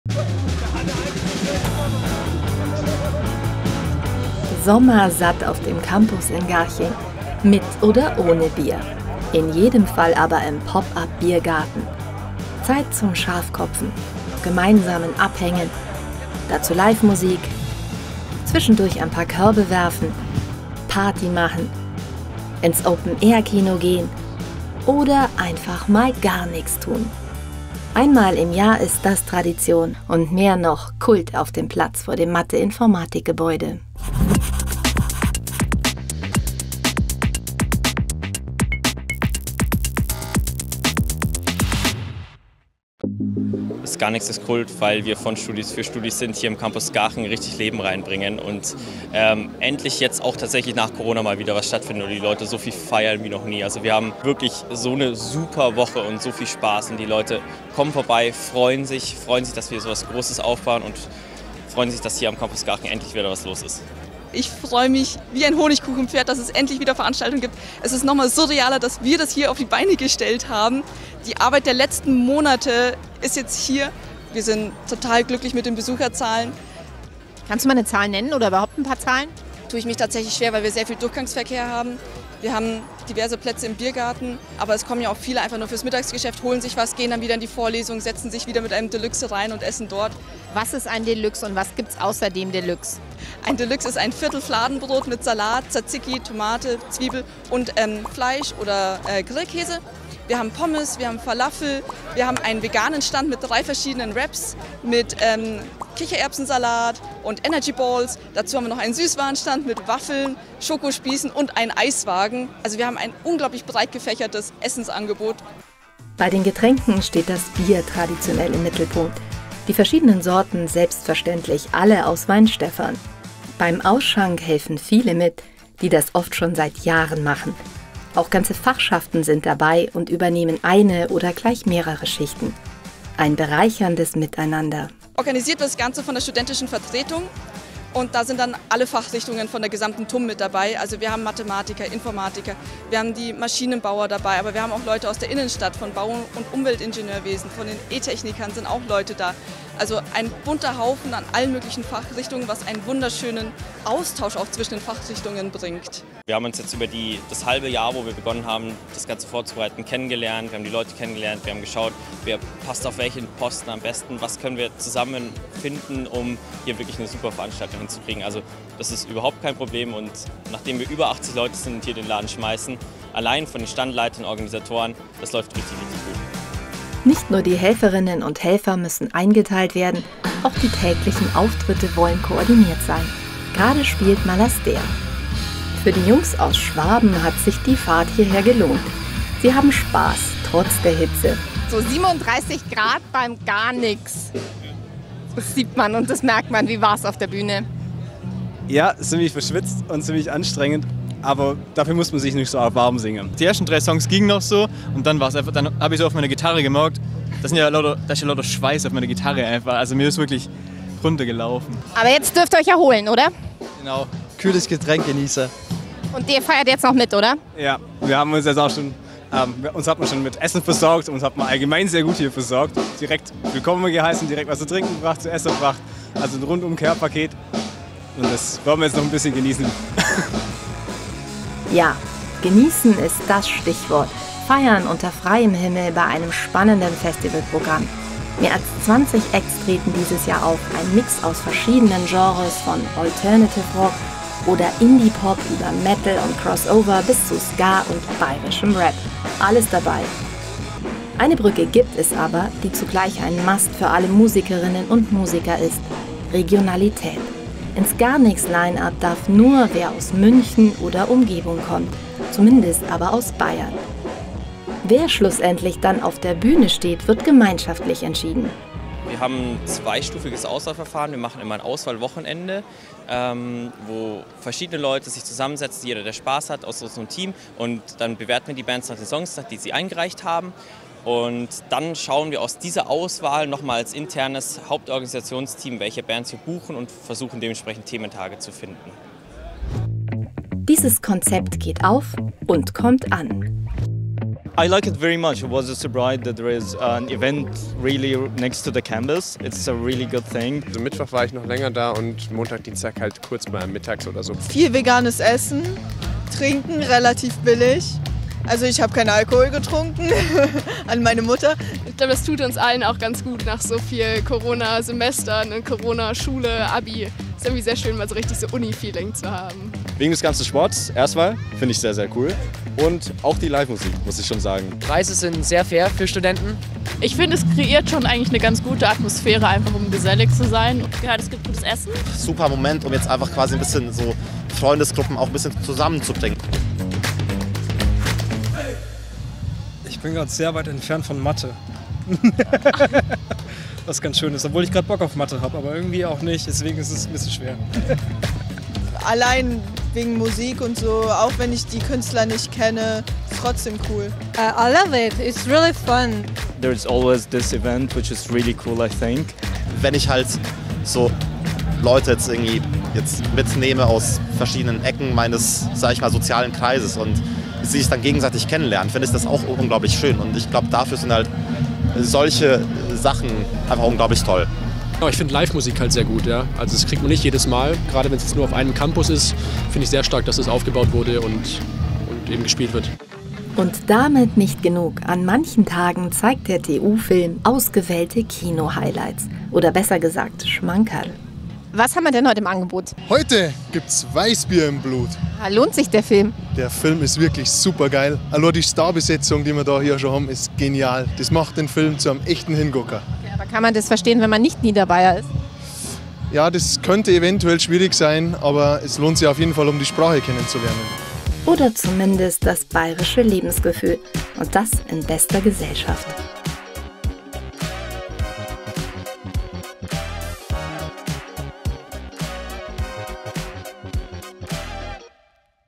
Kult auf dem Campus: GARNIX 2022 | TUMinside
Seit gut 20 Jahren bringt das GARNIX-Festival ordentlich Leben auf den TUM Campus Garching. Mit Biergarten, Bühne und Bands sorgen die Organisatorinnen und Organisatoren der Studentischen Vertretung und ihre unzähligen Helferinnen und Helfer für bestes Sommerfeeling. Aber das GARNIX ist noch viel mehr: Hier gibt es Essen aus aller Welt, Sportangebote, Open-Air-Kino – und vor allem jede Menge Spaß im Kreise der Kommilitoninnen und Kommilitonen.

For around 20 years, the GARNIX Festival has brought plenty of life to the TUM Campus Garching. With beer gardens and bands, the organizers of the Student Council and their countless helpers provide for proper summer feeling. But GARNIX is much more than that: There is food from all over the world, numerous sports activities, open-air cinema – and above all lots of fun with fellow students.

► Folge dem TUM Center for Study and Teaching

► Folge der TUM